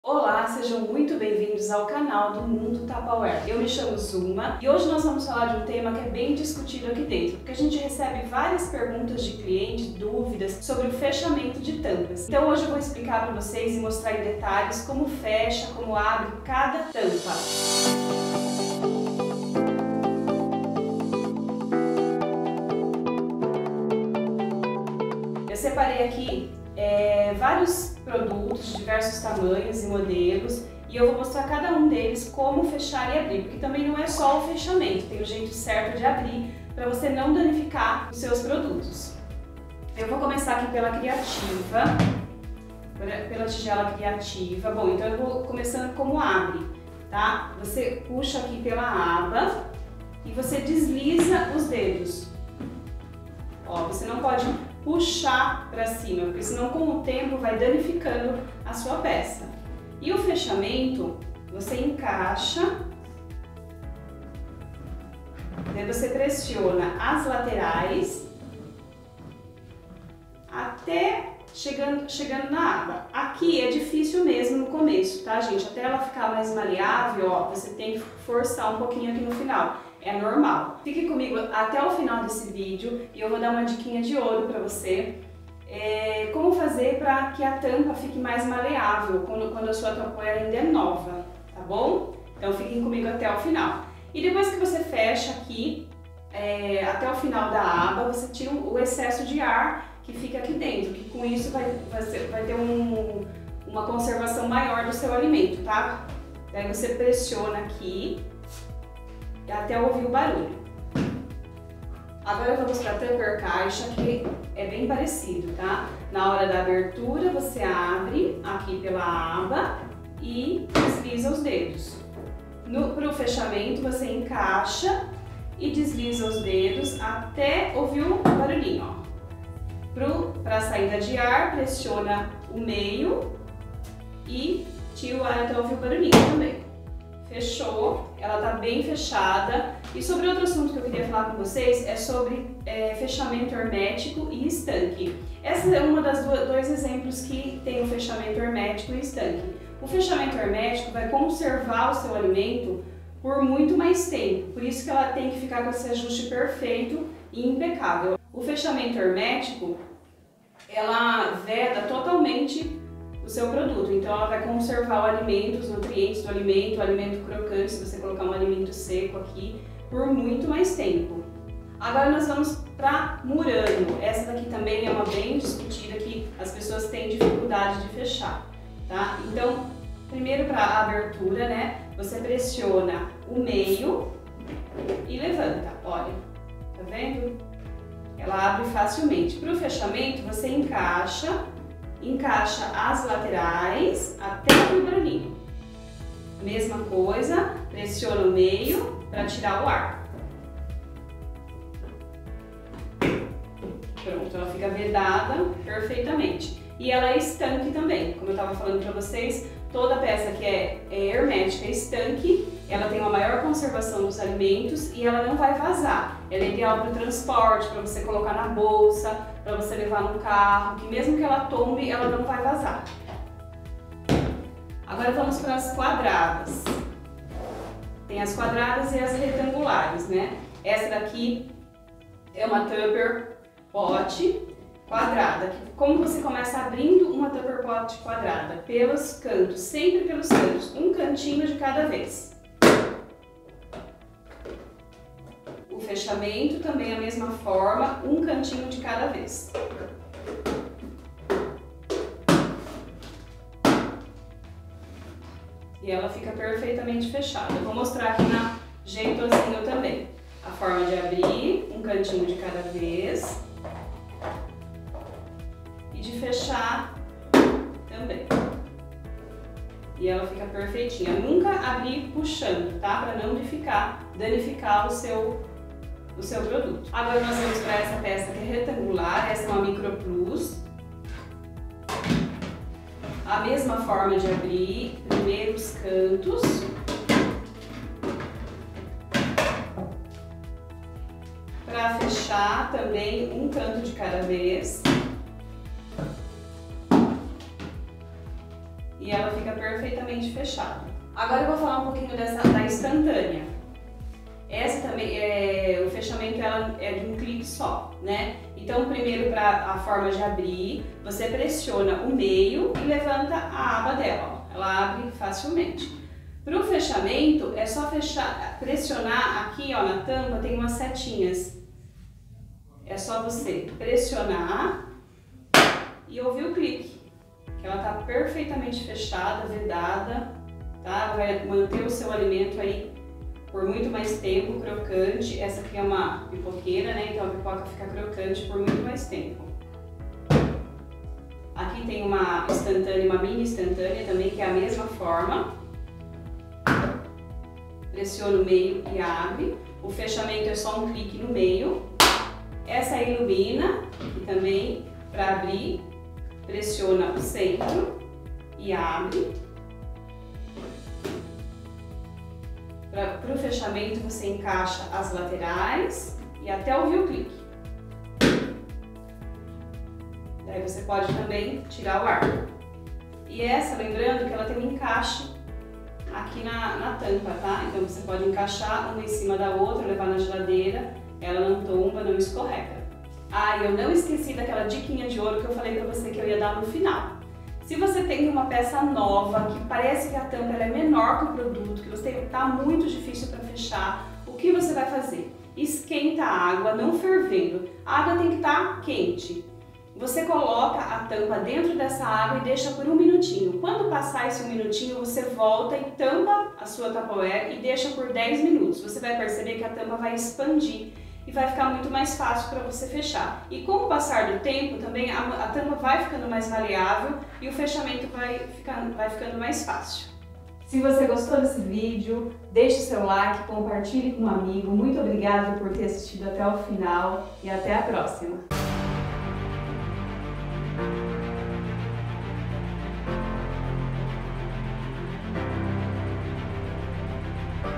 Como Fechar sua Tupperware Corretamente + Dicas
Descubra nesse vídeo os segredinhos do fechamento  perfeito de diferentes tampas da Tupperware.

E uma dica especial para você que tem dificuldade  com o fechamento da sua Tupperware nova.